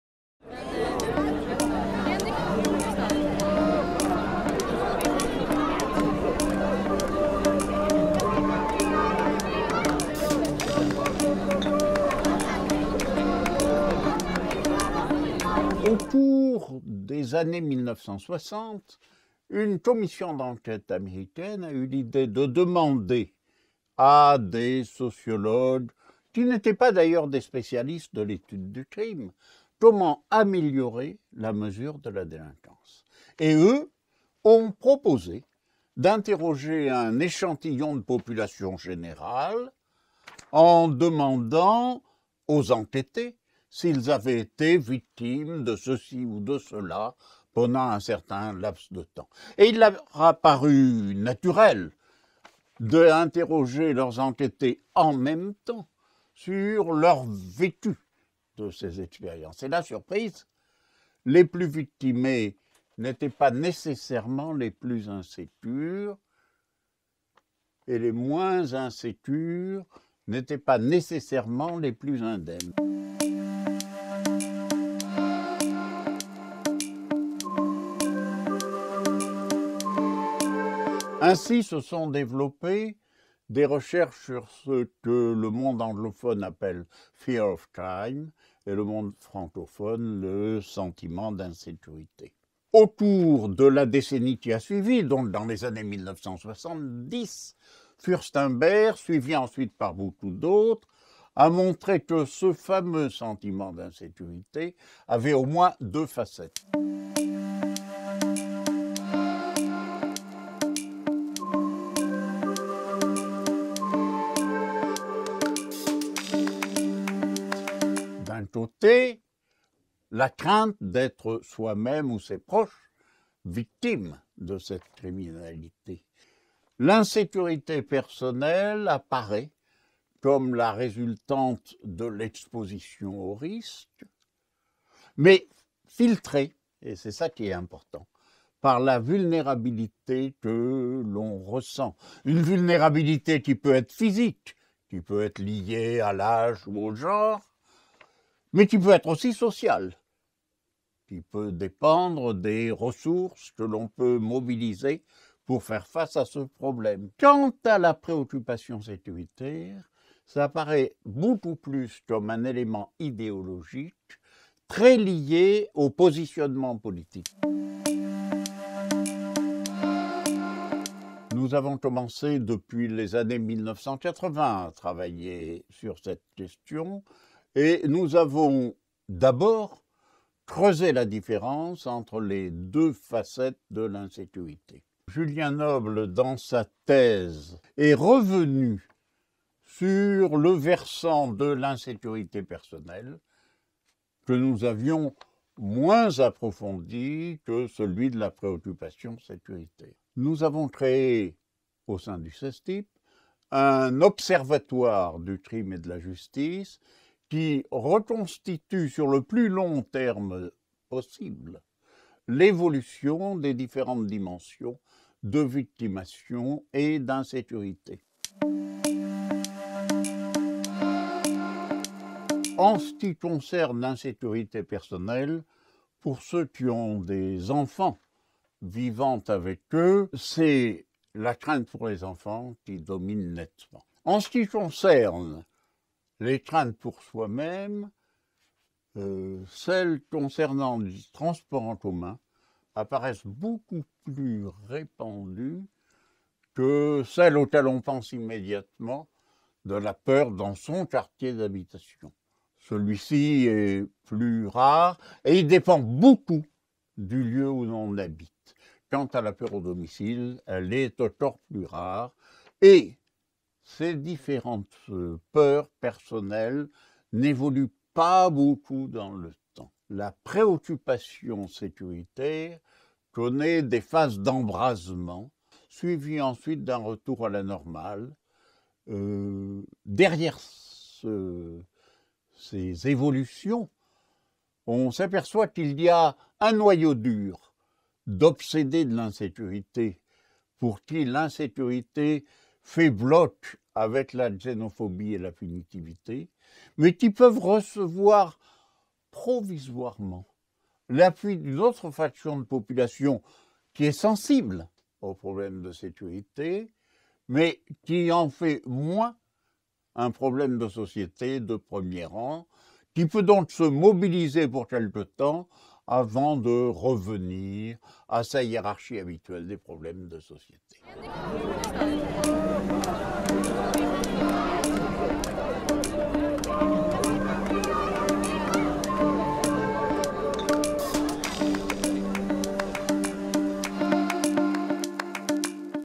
Sentiment d'insécurité, par Philippe Robert
L’importance de la délinquance ne tient pas seulement à sa fréquence, mais aussi à ce qu’en pensent les citoyens. Certains peuvent craindre d’en être victimes mais les plus exposés ne sont pas toujours les plus apeurés. Mais on peut aussi être préoccupé par l’importance de la criminalité comme problème de société, quand bien même on ne se sentirait pas personnellement menacé.

Pour aller plus loin :
Robert Ph., Zauberman R., 2017, Du sentiment d’insécurité à l’Etat sécuritaire, Lormont, Le Bord de l’Eau.
Robert Ph., Zauberman R., 2018, Le sentiment d'insécurité et les politiques de sécurité,
Robert Ph., 2016, V° Insecurity and Fear of Crime (new edition) in Ritzer G. (ed.), The Blackwell Encyclopedia of sociology, Oxford, Blackwell Publishing
Robert Ph., 2018, Insécurité in Savidan P., Dir., Dictionnaire des inégalités et de la justice sociale, Paris, PUF, 788-792